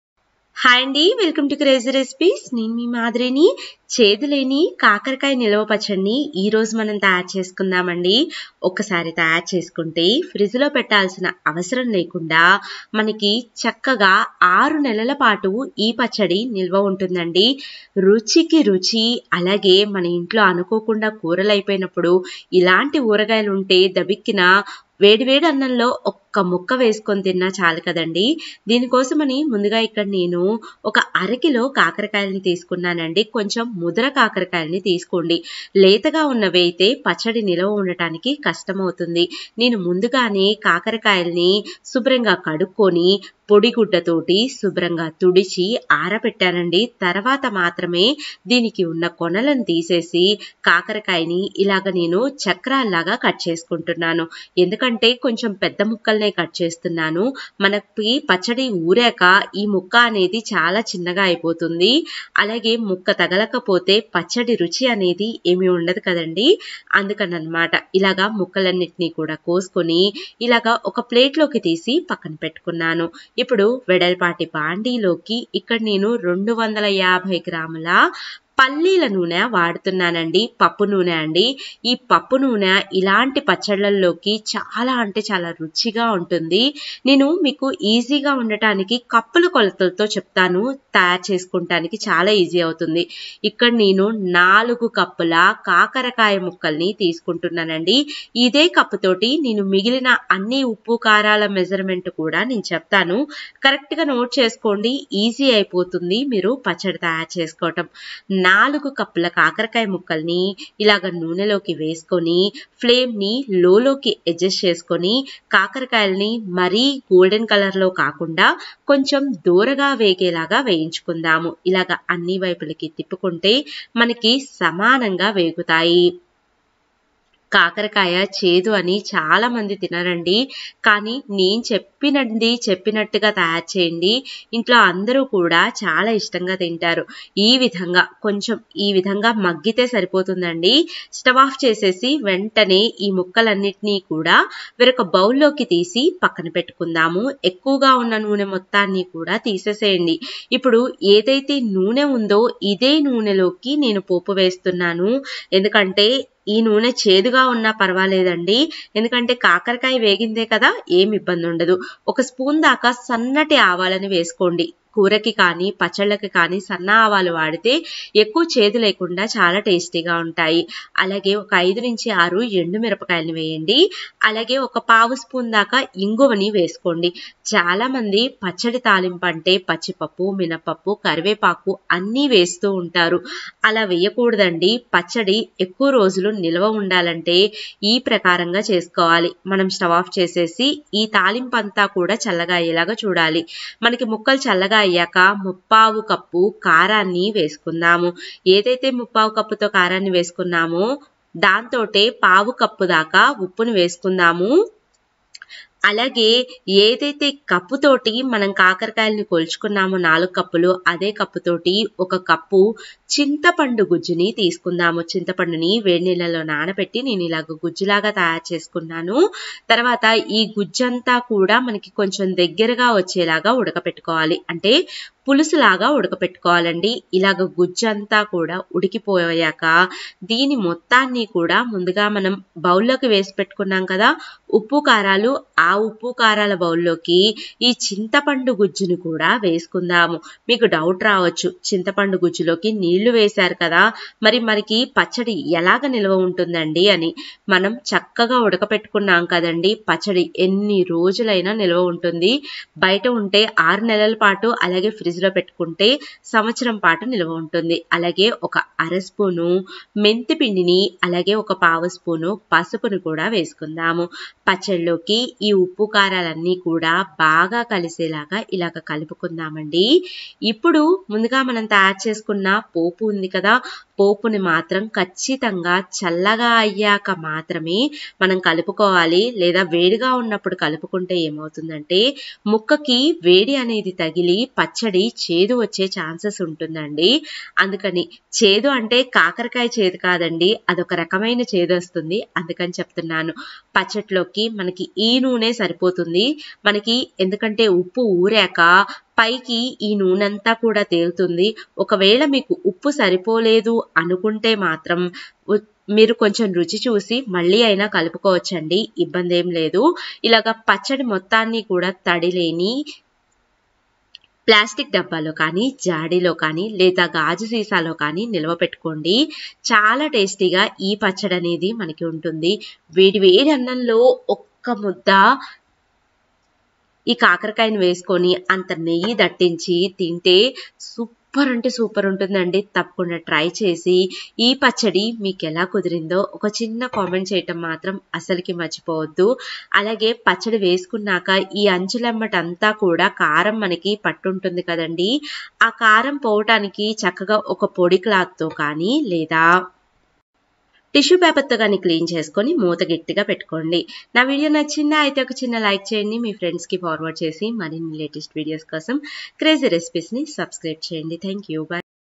కాకరకాయ నిల్వ పచ్చడి తయారీ విధానం | Kakarakaya Nilva Pachadi |Bitter Gourd Pickle
కాకరకాయ నిల్వ పచ్చడి అస్సలు ఎలా చేస్తారో మీకు తెలుసా | Kakarakaya Nilva Pachadi | Kakarakaya Pachadi @crazyrecipes